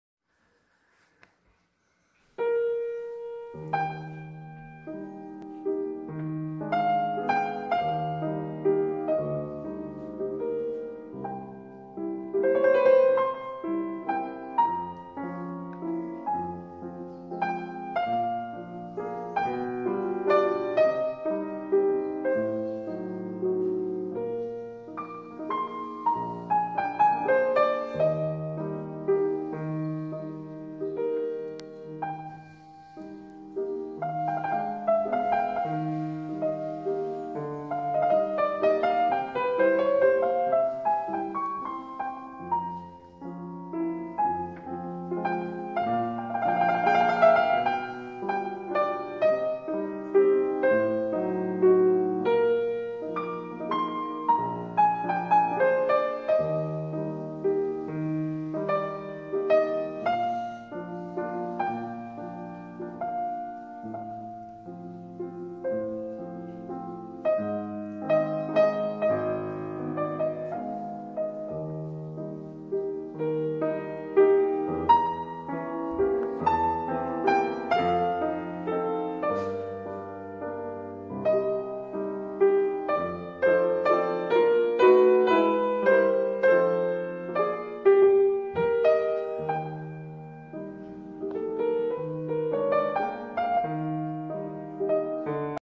KM#2113 EIN KREUZER 1816 A HABSBURG
predstavljanje Austrougarske kovanice od jednog kreuzera (krajcar) iz 1816.

KM 2113

A- wienna